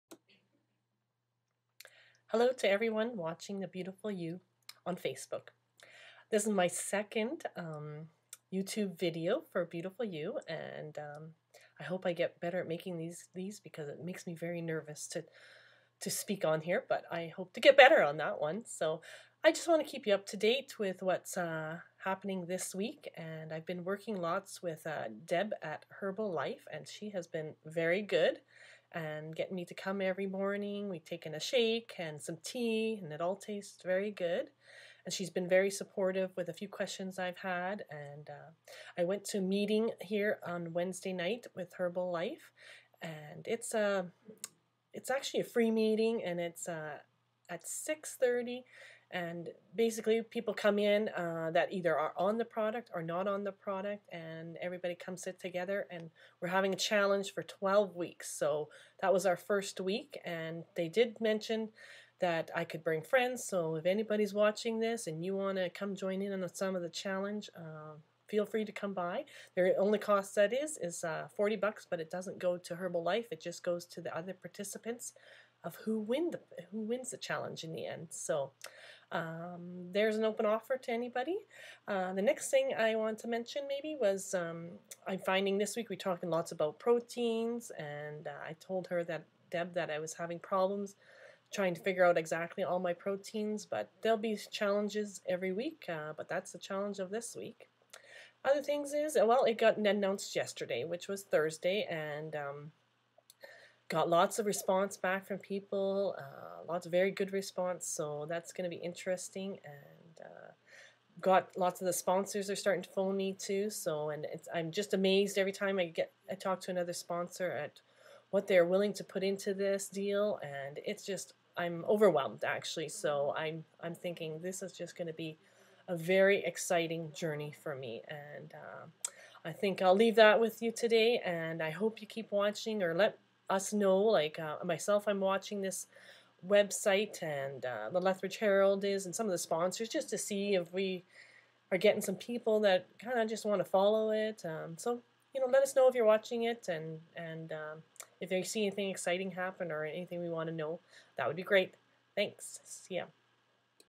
Beautiful You Update
Brenda talks about her initial days as the new Beautiful You...